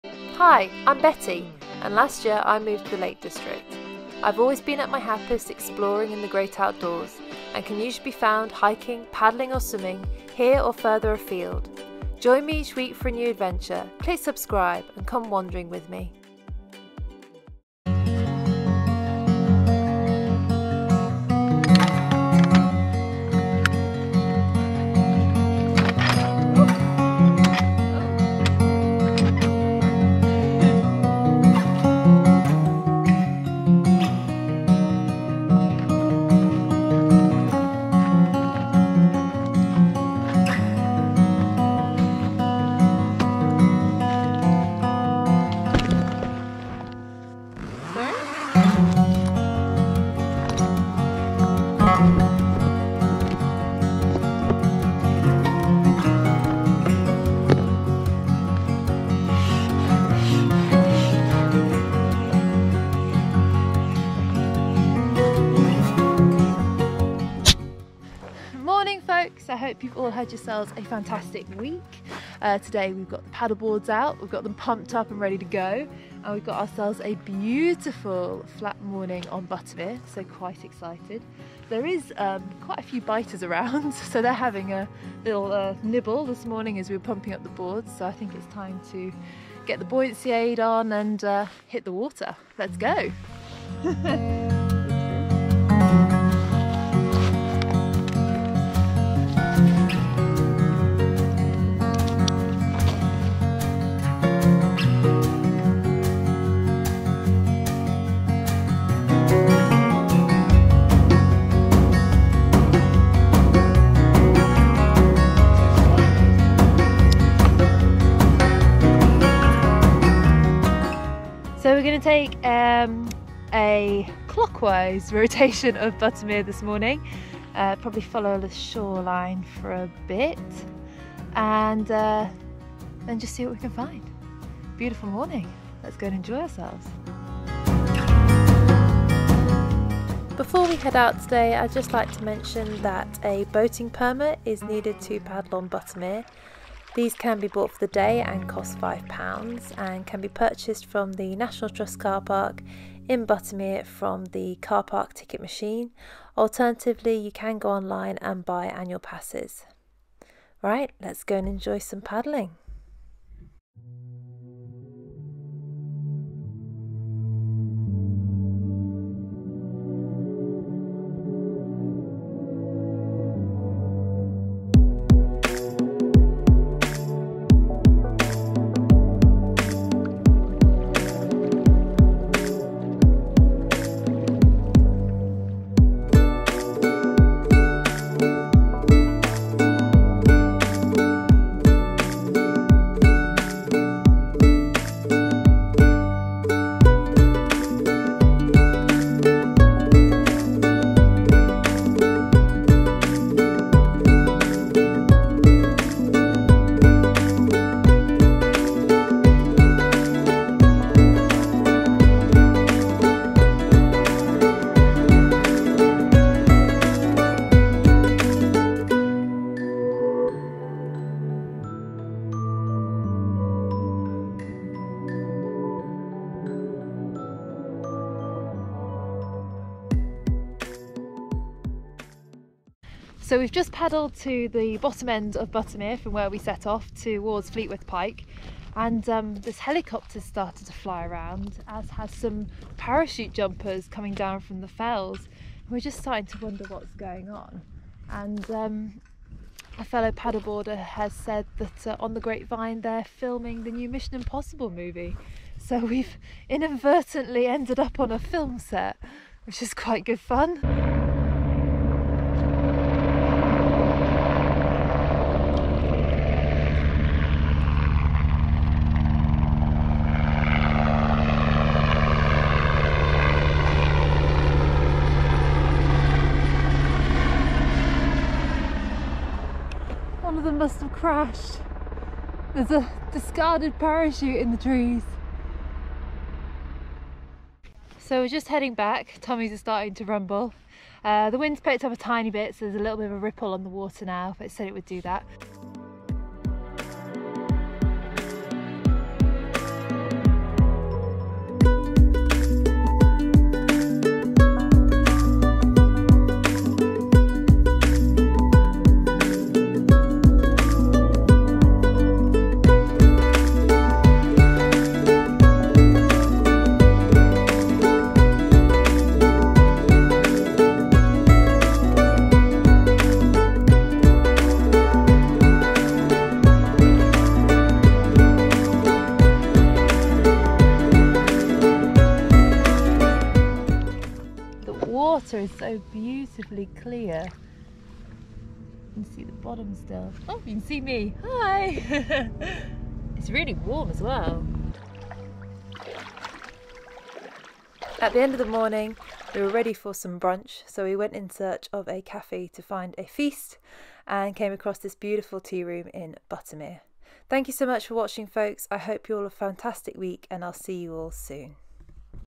Stand Up Paddle Boarding on Buttermere, Lake District
This week we enjoyed a wonderful SUP on Buttermere in the Lake District! We had arrived with our paddle boards in the morning and planned to do a loop of Buttermere before brunch. The calm waters and stunning scenery made for a beautiful paddle and we even bumped into a film crew along the way.

The Lake District has many beautiful places to Stand Up Paddle Board and I look forward to being able to take you along on more paddling adventures. If you have any ideas as to where I should explore next I'd love to hear them in the comments below.